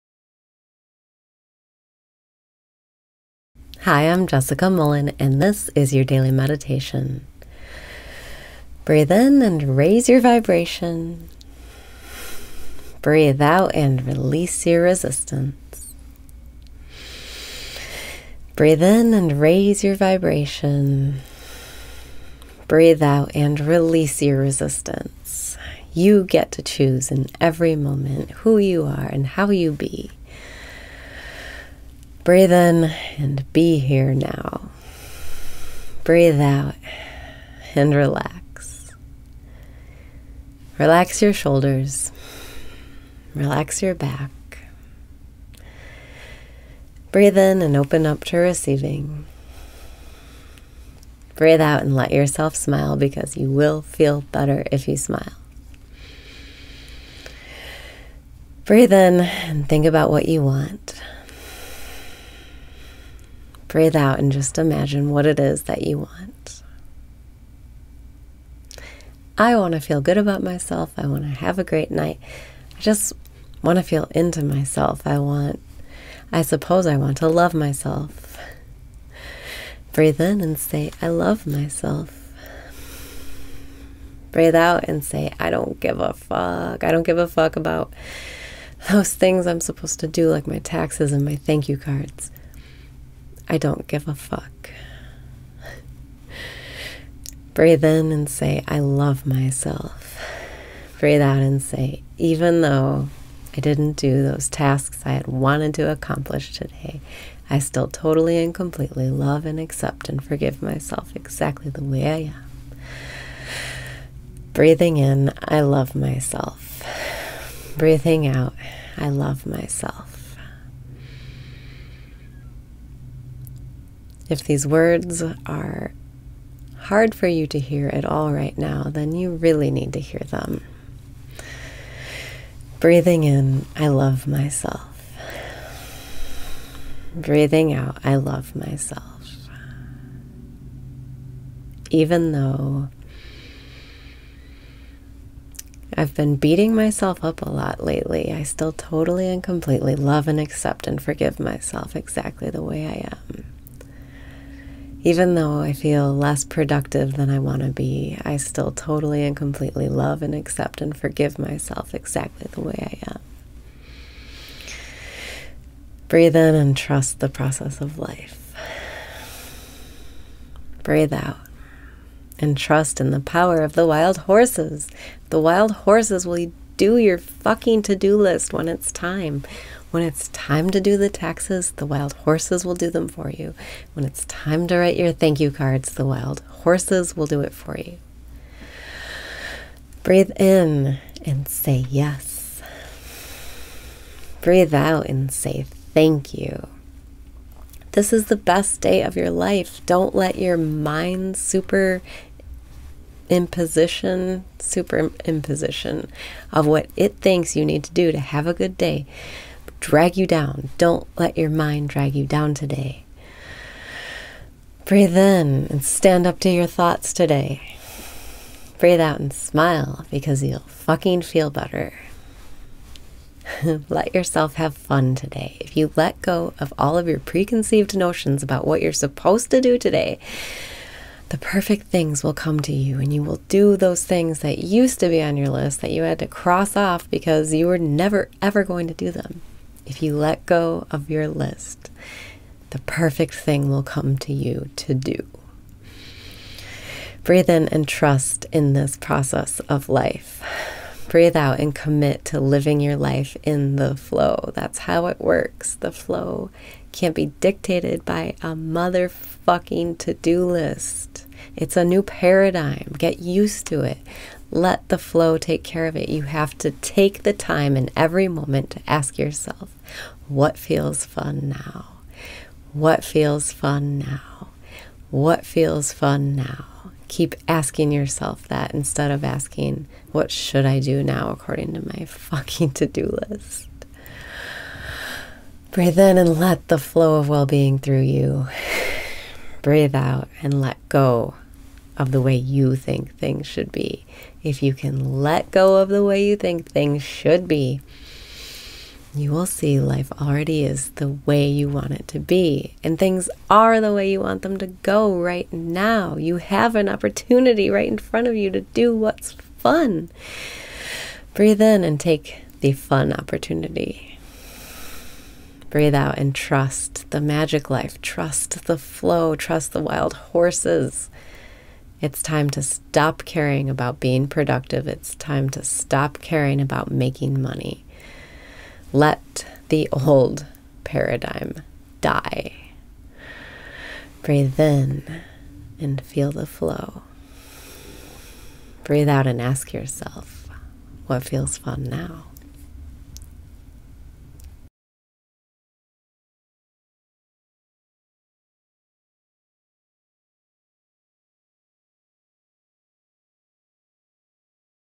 Daily Meditation - Let the Old Paradigm Die
You get to choose in every moment, who you are and how you be.

Relax your shoulders. Relax your back.

Breathe out and let yourself smile because you will feel better if you smile!

Breathe out and just imagine what it is that you want.

I want to feel good about myself. I want to have a great night. I just want to feel into myself. I want—I suppose I want to love myself!

I don’t give a fuck about those things I’m supposed to do like my taxes and my thank you cards. I don’t give a fuck!

Breathe out and say, “Even though I didn’t do those tasks I wanted to accomplish today, I still totally and completely love and accept and forgive myself exactly the way I am.”

If these words are hard for you to hear at all right now, then you really need to hear them.

Even though I’ve been beating myself up a lot lately, I still totally and completely love and accept and forgive myself exactly the way I am. Even though I feel less productive than I want to be, I still totally and completely love and accept and forgive myself exactly the way I am.

Breathe out and trust in the power of the wild horses!

The wild horses will do your fucking to do list when it’s time! When it’s time to do the taxes, the wild horses will do them for you. When it’s time to write your thank you cards, the wild horses will do it for you.

This is the best day of your life! Don’t let your mind’s superimposition of what it thinks you need to do to have a good day drag you down. Don’t let your mind drag you down today!

Breathe in and stand up to your thoughts today!
Breathe out and smile because you’ll fucking feel better!

Let yourself have fun today! If you let go of all of your preconceived notions about what you’re supposed to do today, the perfect things will come to you and you will do those things that used to be on your list that you had to cross off because you were never ever going to do them. If you let go of your list, the perfect thing will come to you to do!

Breathe in and trust in this process of life!
Breathe out and commit to living your life in the flow!

That’s how it works. The flow can’t be dictated by a motherfucking to do list! It’s a new paradigm. Get used to it! Let the flow take care of it! You have to take the time in every moment to ask yourself, “What feels fun now? What feels fun now? What feels fun now?” Keep asking yourself that instead of asking, “What should I do now, according to my fucking to do list?”

Breathe in and let the flow of well being through you!
Breathe out and let go of the way YOU think things should be.

If you can let go of the way you think things should be, you will see life already is the way you want it to be. And things are the way you want them to go, right now. You have an opportunity right in front of you to do what’s fun!

Breathe in and take the fun opportunity!
Breathe out and trust the magick life!

Trust the flow! Trust the wild horses! It’s time to stop caring about being productive. It’s time to stop caring about making money. Let the old paradigm die!

Breathe in and feel the flow!
Breathe out and ask yourself, “What feels fun now?”